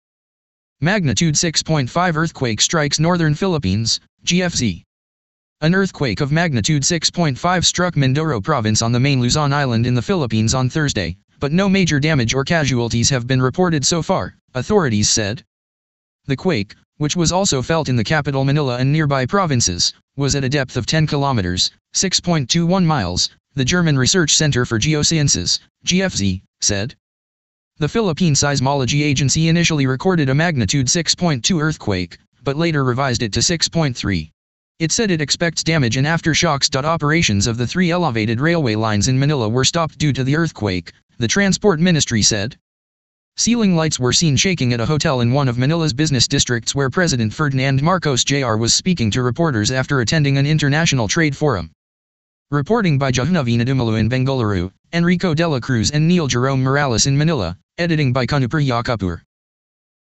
Magnitude 6.5 earthquake strikes northern Philippines
An earthquake of magnitude 6.5 struck Mindoro province on the main Luzon island in the Philippines on Thursday, but no major damage or casualties have been reported so far, authorities said.

The quake, which was also felt in the capital Manila and nearby provinces, was at a depth of 10 kilometres (6.21 miles), the German Research Centre for Geosciences (GFZ) said.